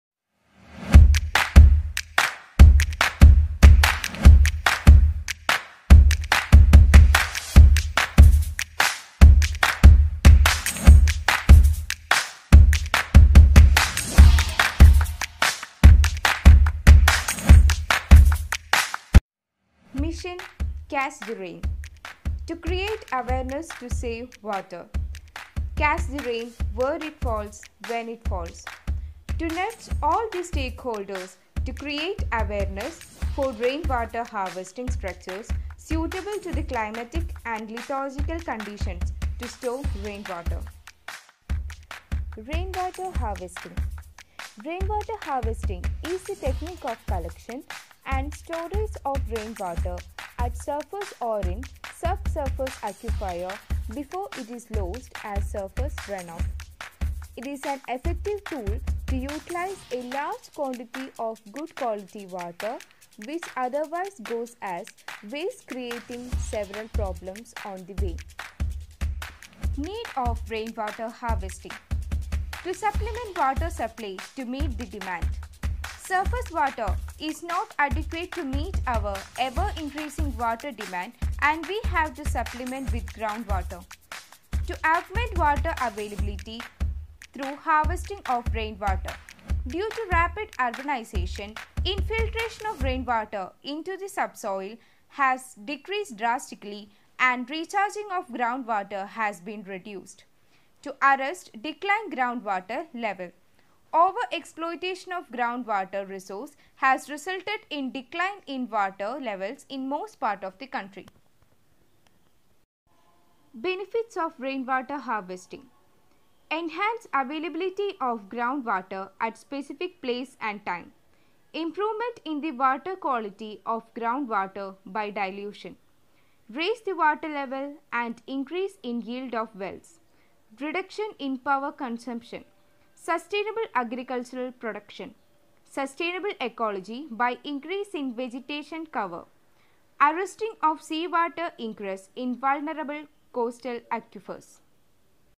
Catch the Rain...
As India is gearing up for the celebration of its *75th years of India’s Independence* , as a part of commemoration; series of events namely *Azadi ka Amrut Mahotsav* have been planned for their organization across the country. It is an initiative to celebrate and commemorate 75 years of progressive India and its glorious history, people, culture and achievements.The celebration is marked by three events
1) *CATCH THE RAIN* -8th August 2021
2) *FIT INDIA FREEDOM RUN CAMPAIGN* - 13 th August 2021
3) *GREEN VILLAGE CLEAN VILLAGE -* 2nd October 2021
On *August 8th* we are celebrating the first event *CATCH THE RAIN* .As part of this event we ,the *NSS* volunteers of *LISSAH COLLEGE* did some activities about the *importance of water conservation and Rain water harvesting.